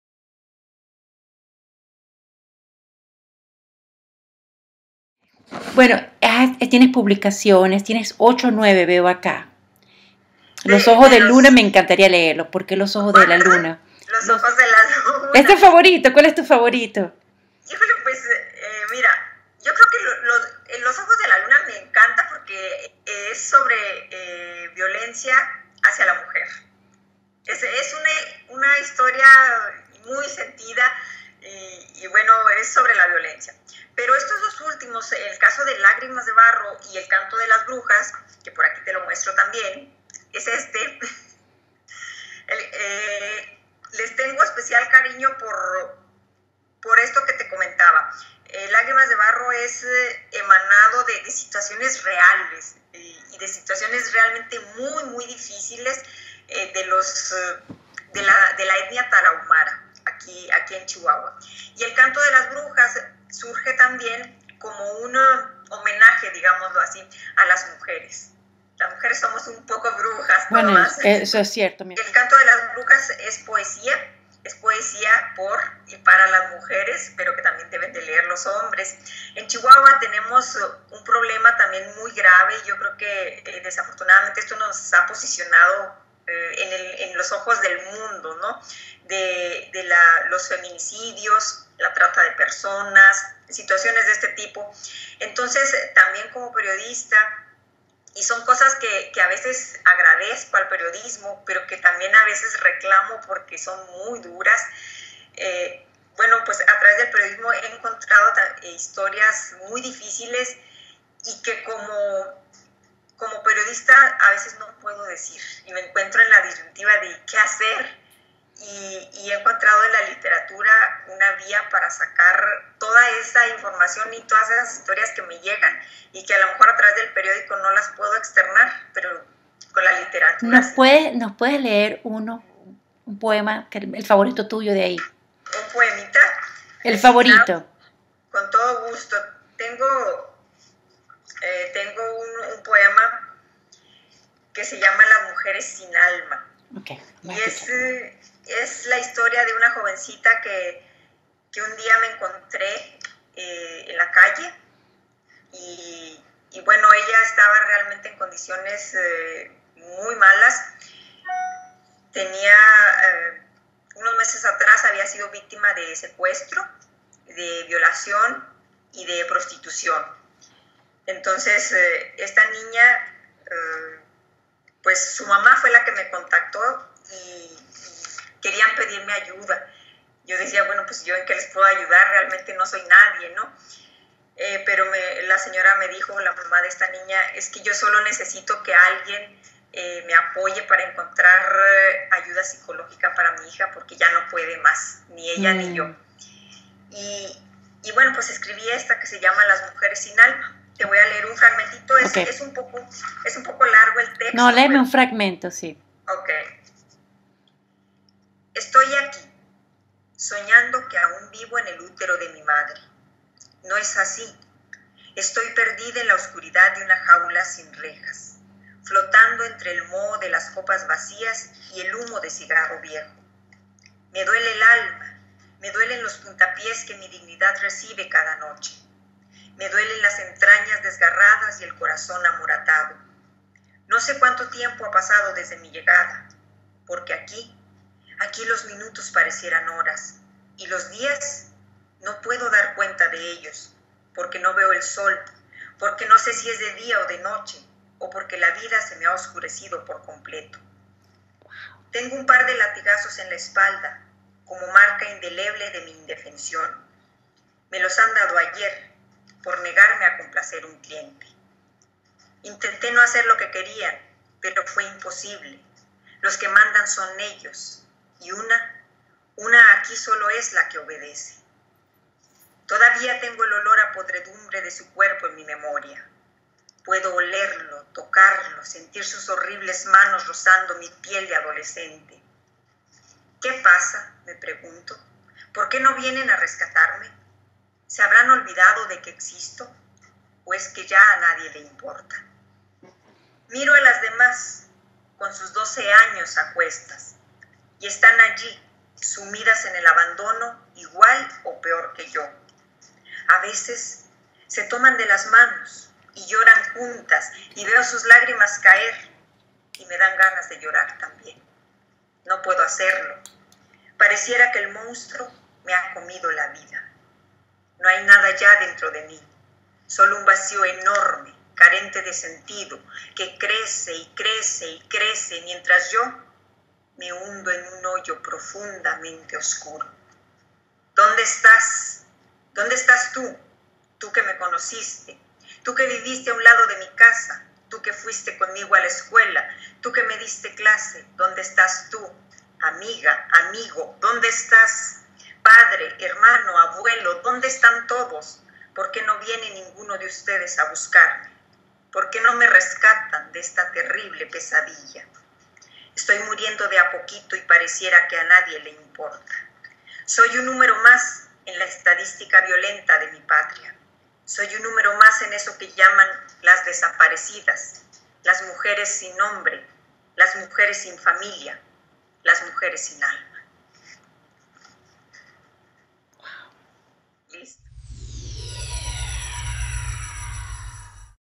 Salud Ochoa Sánchez: Poema LAS MUJERES SIN ALMA
LAS MUJERES SIN ALMA es el poema de SALUD OCHOA SÁNCHEZ en su último libro de poesía EL CANTO DE BRUJAS.  SALUD es una premiada periodista, autora, conferencista y poeta de Chihuahua, México.  Salud nos recita un poema  inspirado el historia real de una joven víctima de secuestro, violación y prostitución. Entrevistada por Lorena Mora-Mowry para Mujer Latina Today.